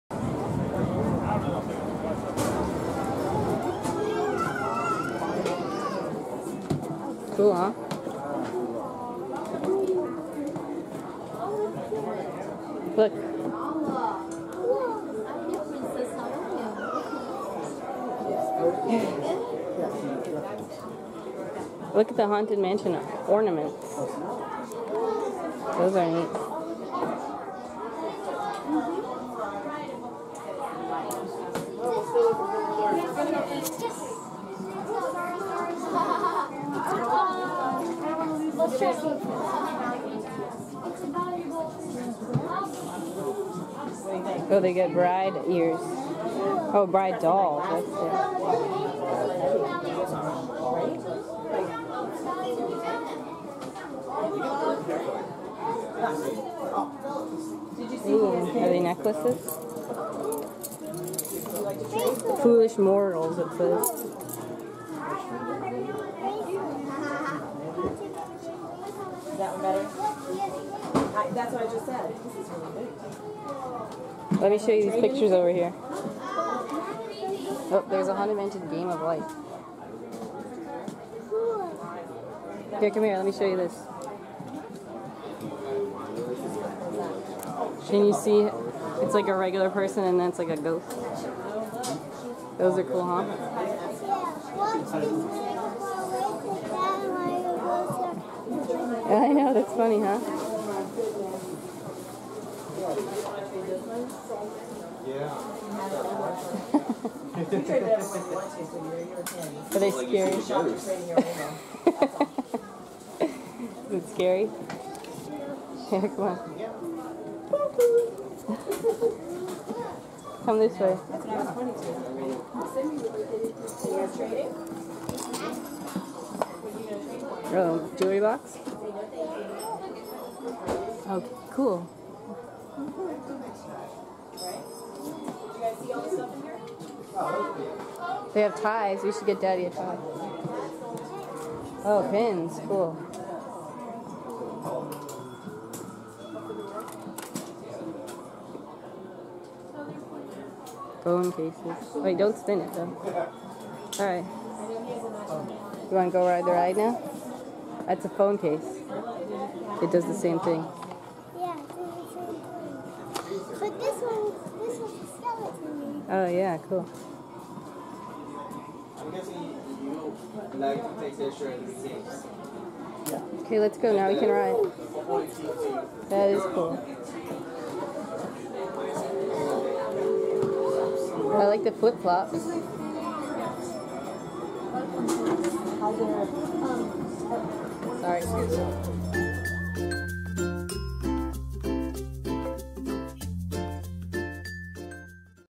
Disney World Souvenir Hunt at Memento Mori - Haunted Mansion Merchandise
Scarlett checks out the Haunted Mansion themed souvenirs available at Momento Mori in Disney World's Magic Kingdom. The portraits that change from normal to ghost are freaky!